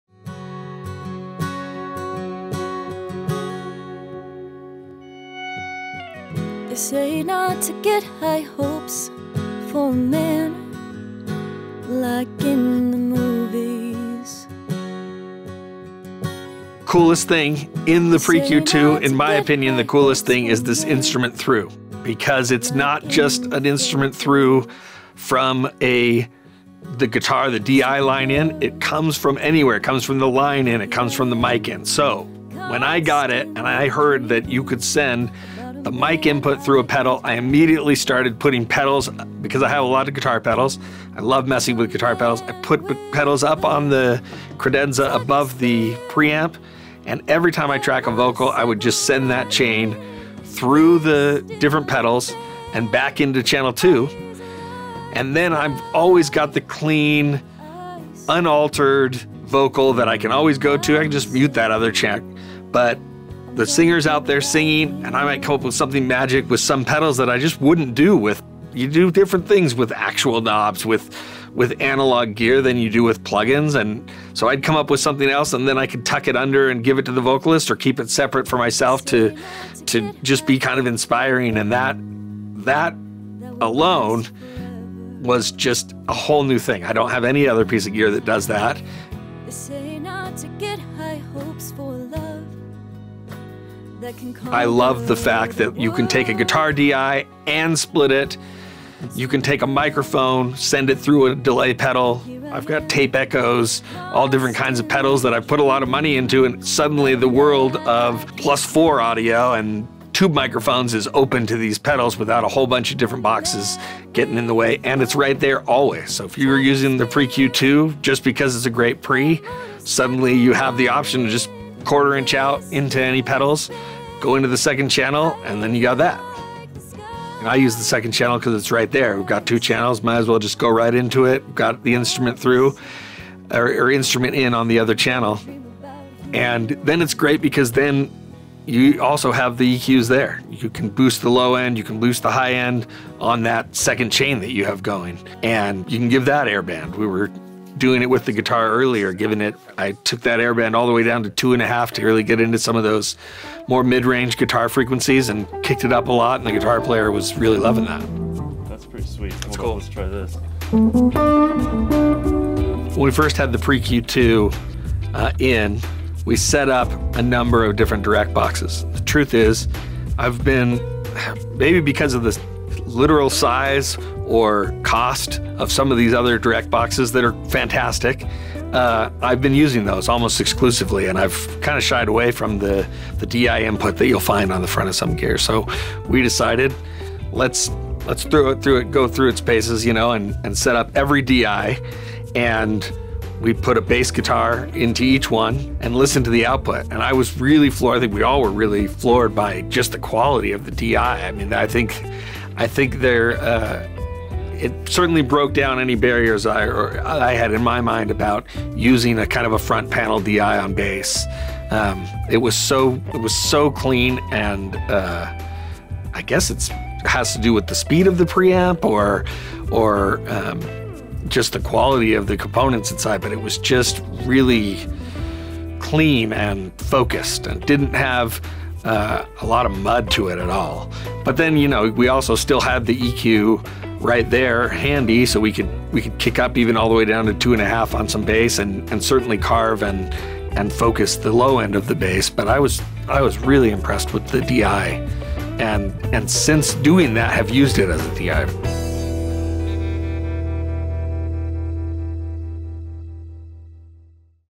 INSTRUMENT / MIC INPUT: Mäag Audio PREQ2 Dual Mic Preamp with EQ
Owner/Engineer/Producer at the state of the art June Audio Recording Studios, Scott Wiley shares his experience with the MaagPREQ2.